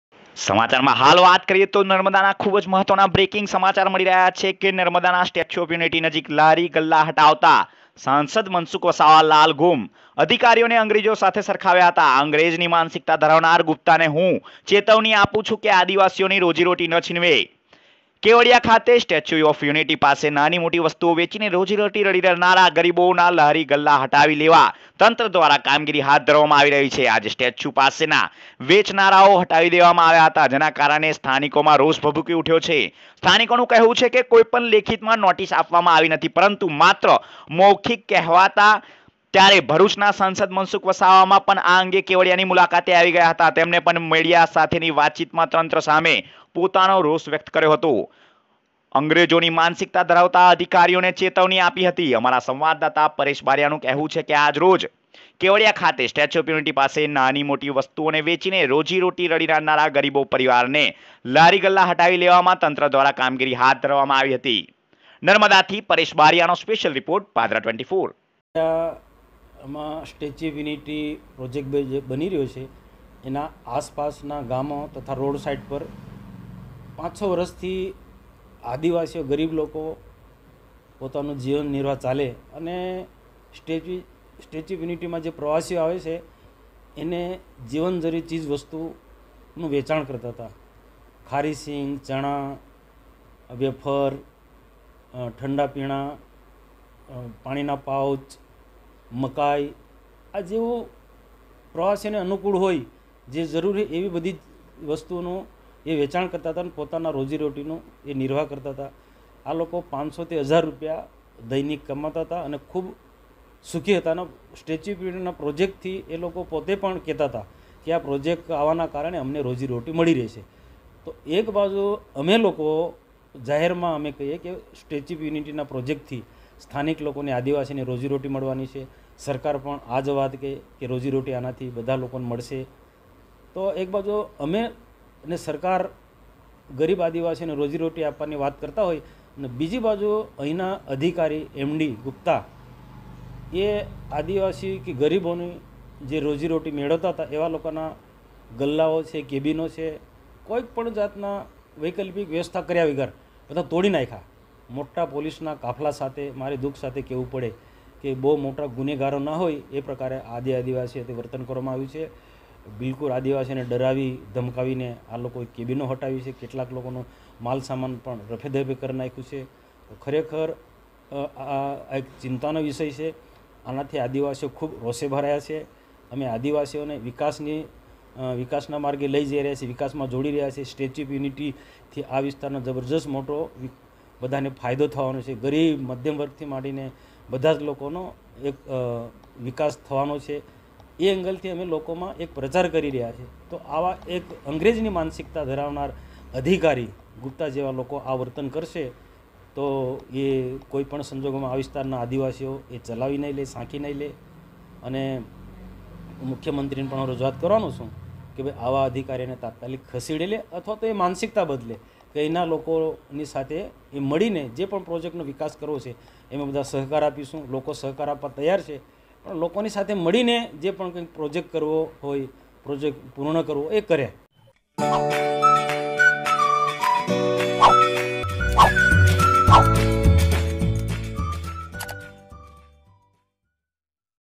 સ્ટૅચ્યૂ ઓફ યુનિટી નજીક લારી-ગલ્લા હટાવાતાં સાંસદ મનસુખ વસાવા લાલઘૂમ.
સ્ટૅચ્યૂ ઓફ યુનિટી નજીક લારી-ગલ્લા હટાવાતાં સાંસદ
મનસુખ વસાવા લાલઘૂમ.